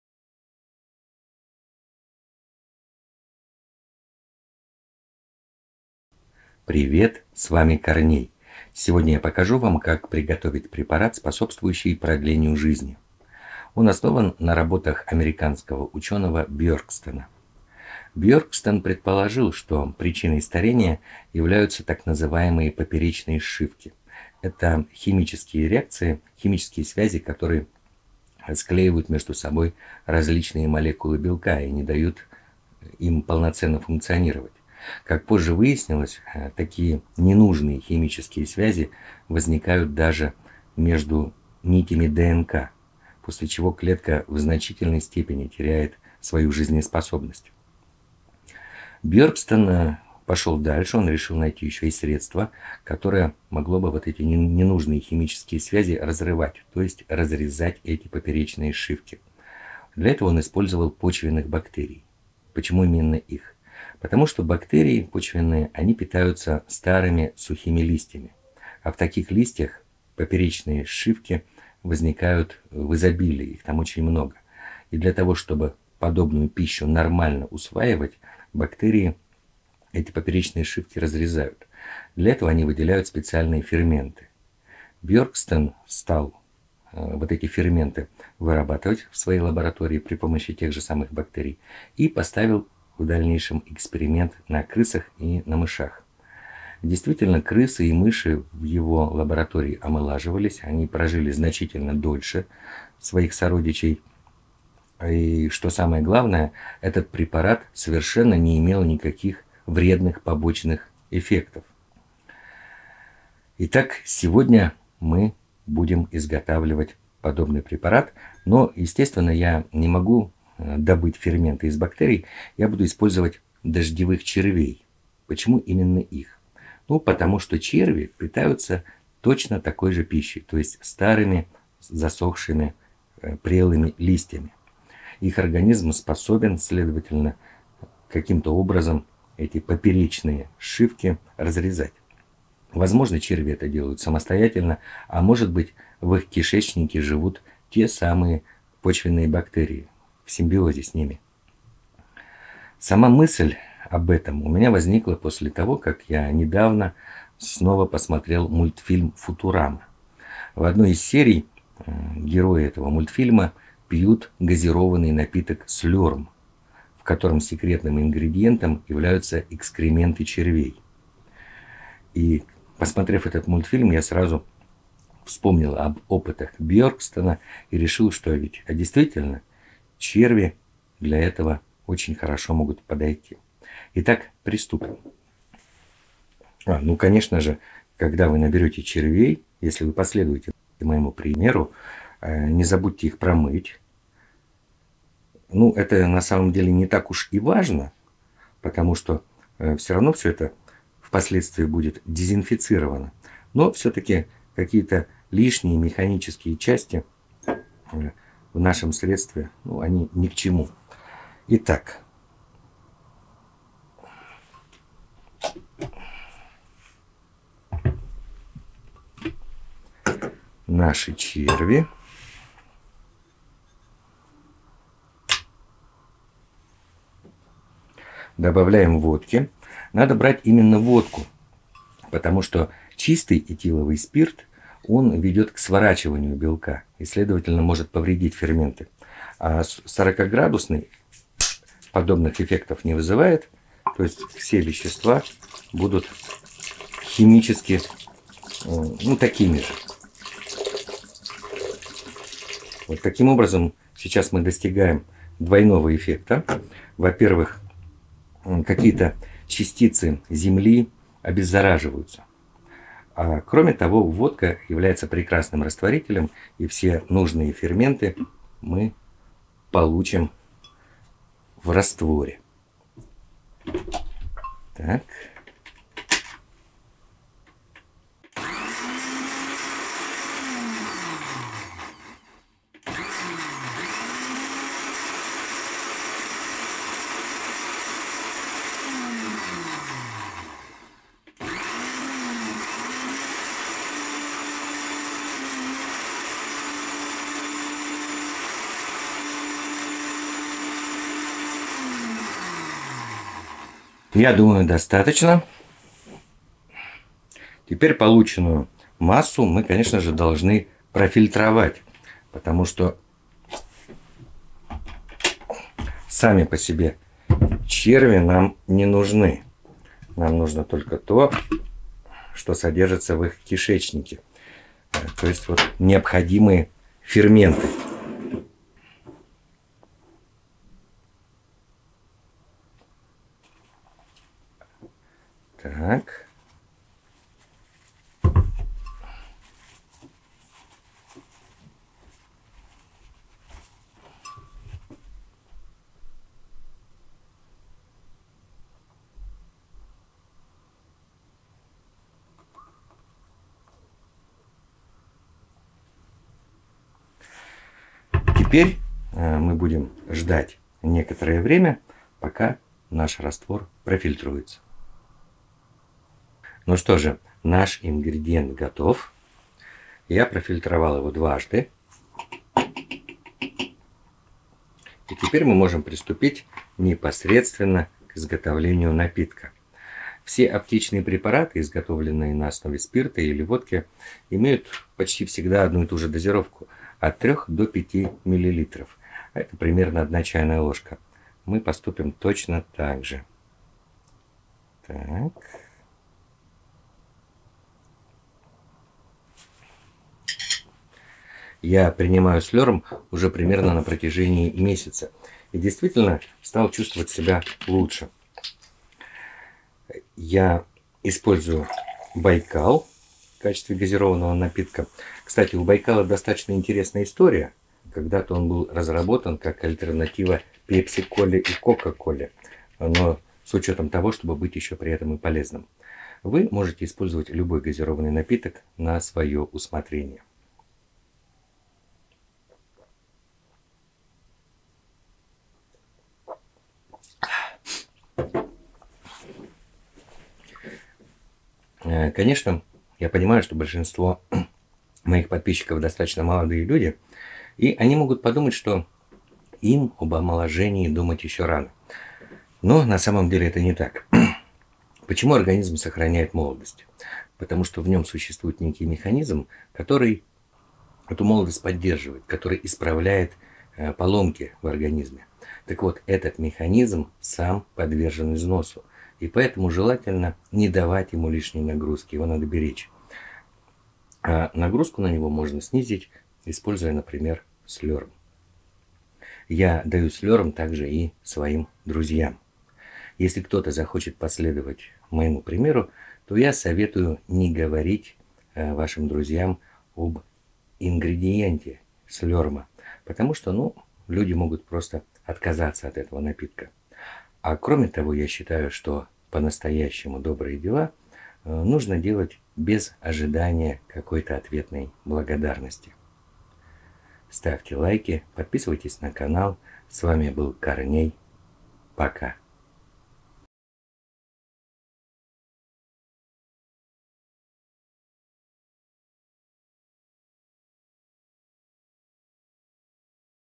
Как сделать эликсир бессмертия #2 (Слёрм)
Эликсир бессмертия на основе научных работ американского учёного Бьёркстена. Подлинное бессмертие скорее всего не достижимо, однако данный эликсир бессмертия может существенно продлить жизнь. В рецепте также использованы некоторые идеи из мультфильма Футурама (напиток Слёрм).

Прогресс неизбежен!